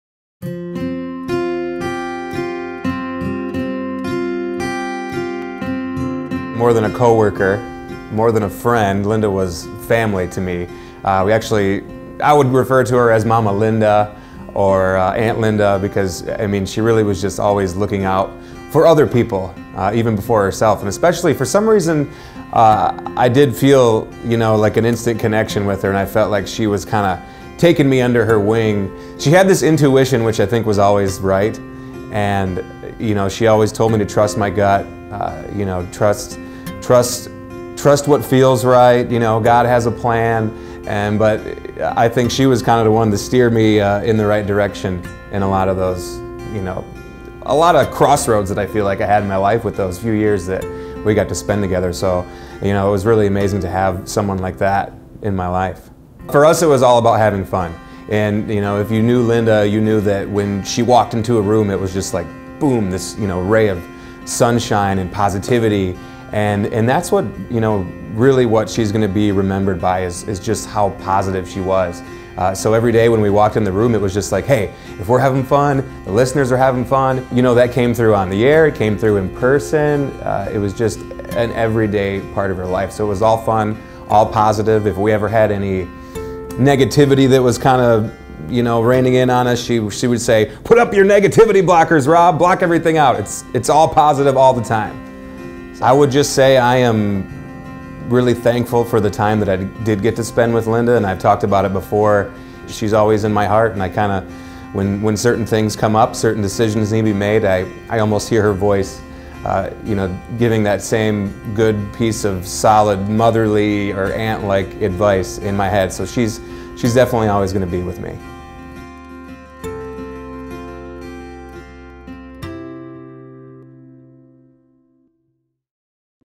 CBS Radio Remembers Country Radio Hall Of Fame Inductee Linda Lee
Linda Lee was posthumously inducted into the Country Radio Hall Of Fame in 2017. The CBS Radio family took a moment to share stories and what was unique about this legendary Detroit radio broadcaster. Hear Linda's most recent radio partner and current WYCD Afternoon Drive host Rob Stone. Linda Lee died after a battle with lung cancer in 2017.